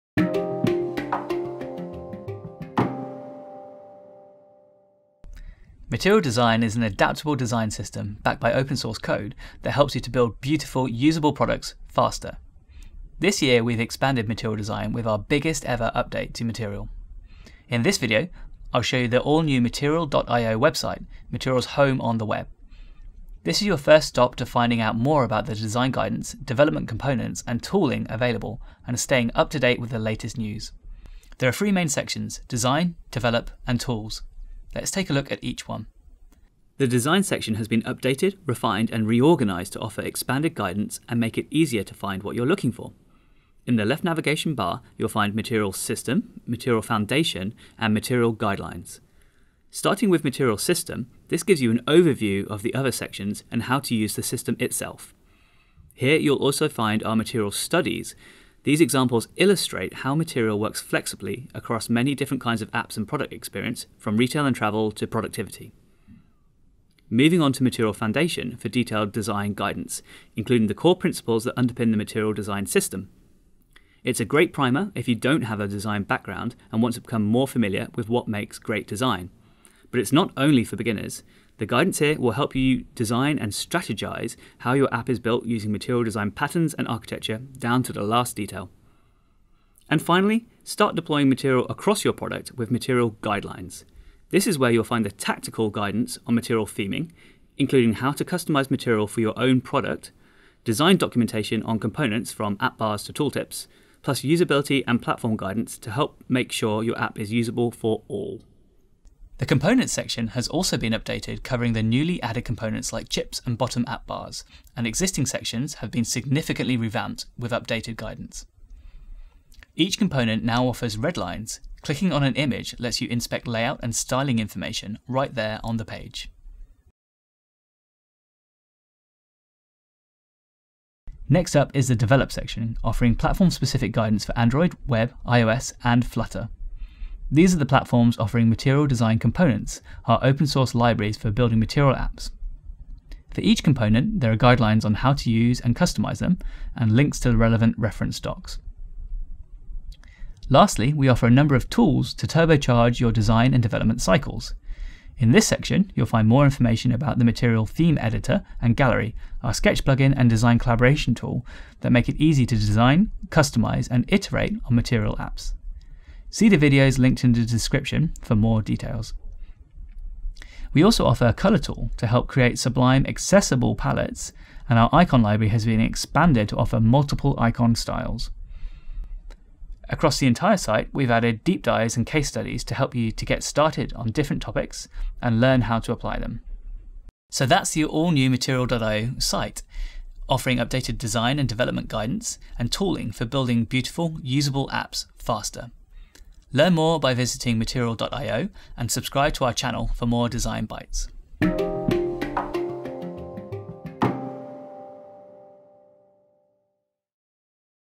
The New Material.io
Discover what’s new with Material.io with a walk through of the new Design, Develop, and Tools landing pages. Learn how to access our Tools and Material Studies available to designers and developers.

Follow us @materialdesign